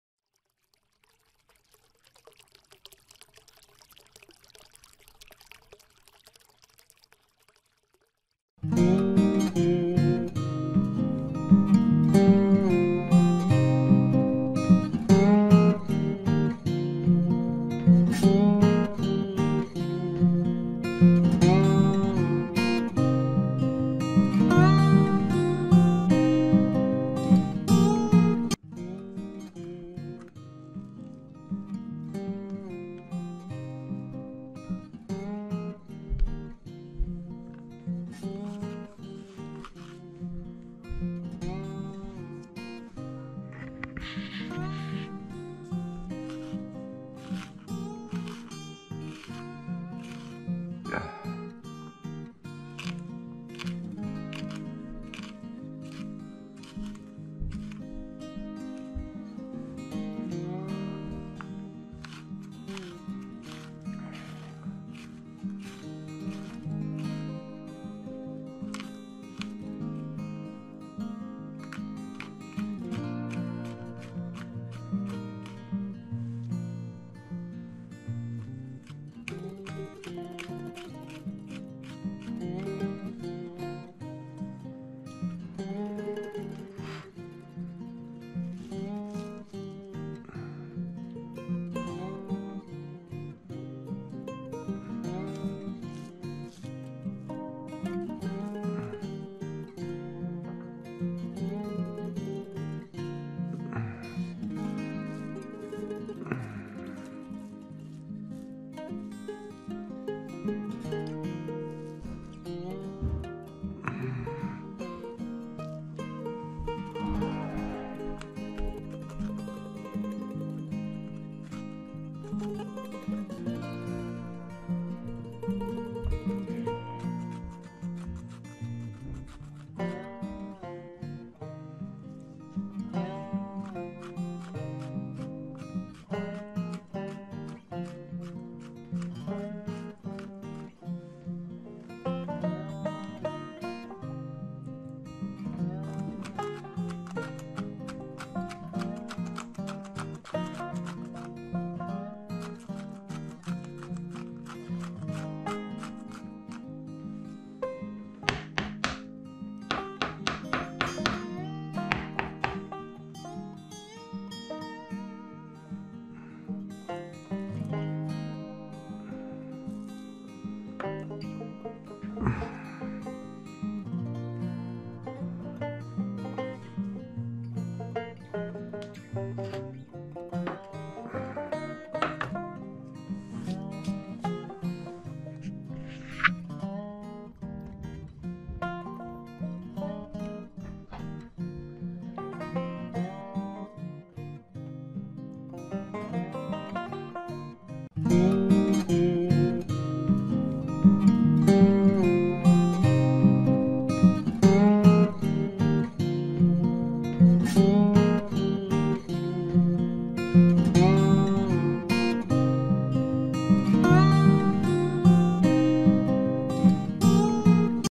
きんぴらネタ作り
ブロッコリーの固くて食べられない芯

スライスしてきんぴらのネタを作ります。
スライスできない残りは刻んできんぴら。

キャベツなどの芯も同様に。

設備屋の適当田舎暮らし